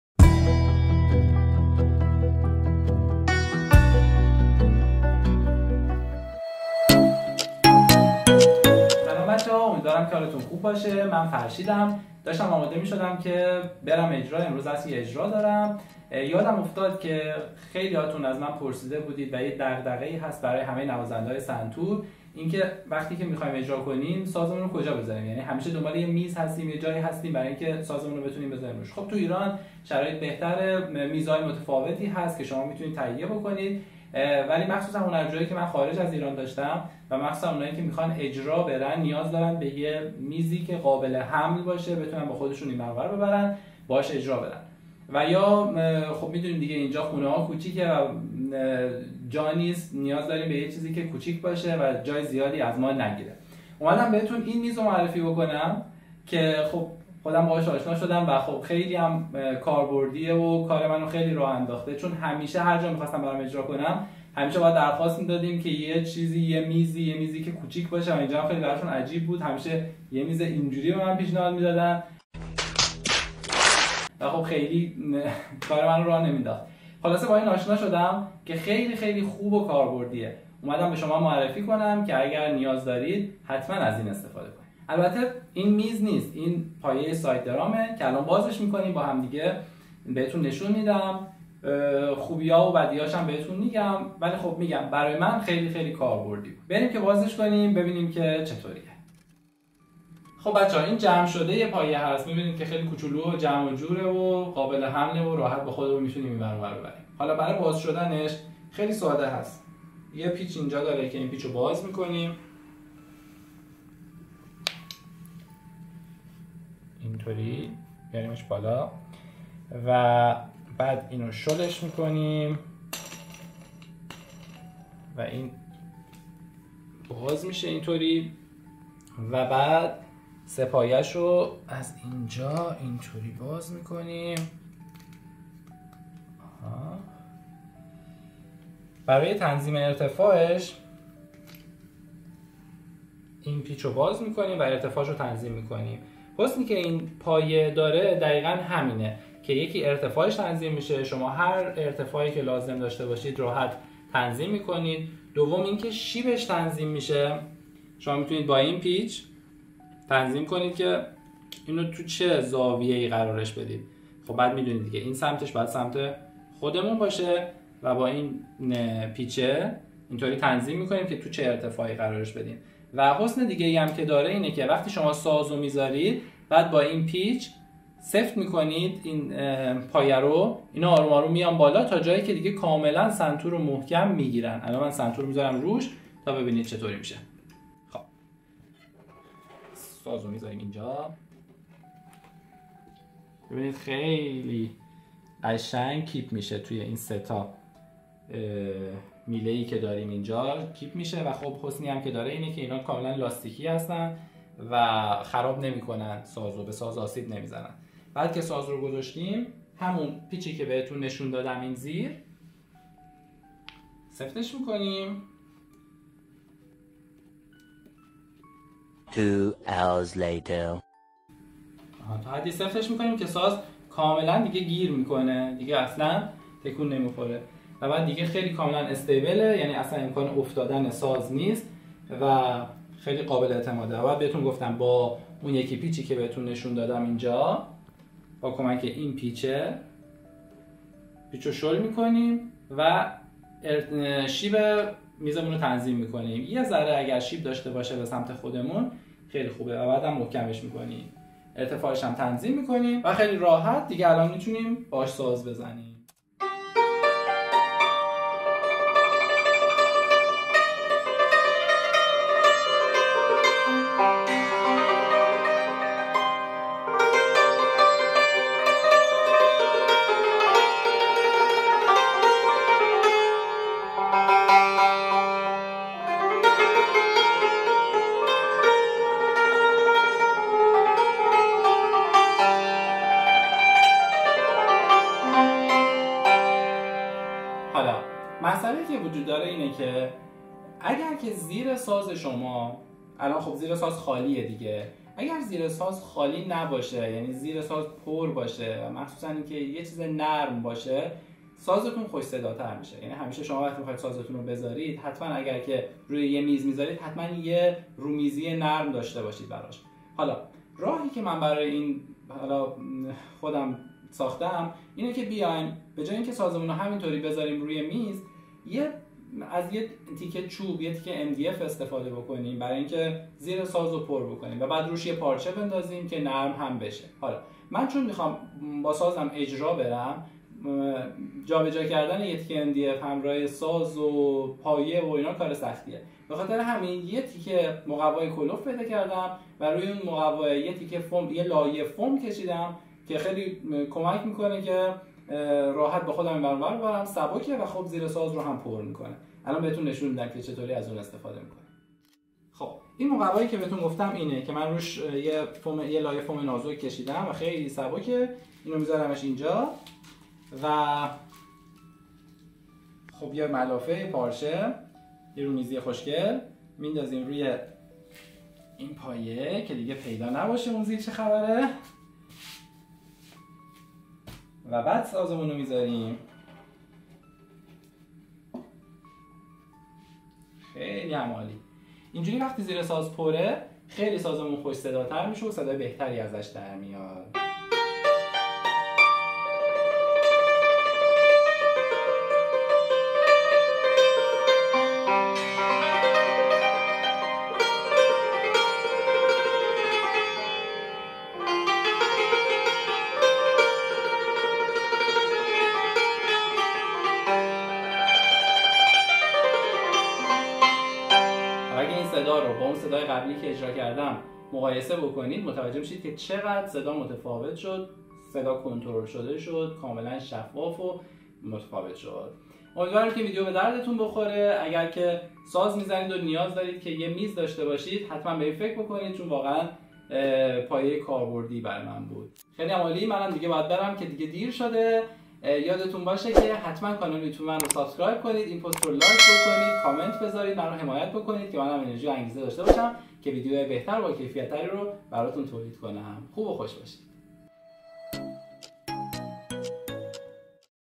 چه میز سنتوری بگیریم که سبک و قابل حمل باشه؟؟
نکته خیلی مهم در اجرا ، داشتن میز سنتور مناسب و قابل حمل هست . تو این ویدیو یکی از بهتریم میزهای سنتور که سبک و قابل حمل هست و قیمت مناسبی هم داره رو بهتون معرفی کردم ، امیدوارم که براتون مفید باشه .

با استفاده از لینک دونیت زیر، شما میتونید من رو حمایت کنید تا بتونم ویدیوهای بهتر و بیشتری براتون آماده کنم. 🌹

برای هماهنگی کلاس های آنلاین و حضوری در پاریس ، با من در ارتباط باشید :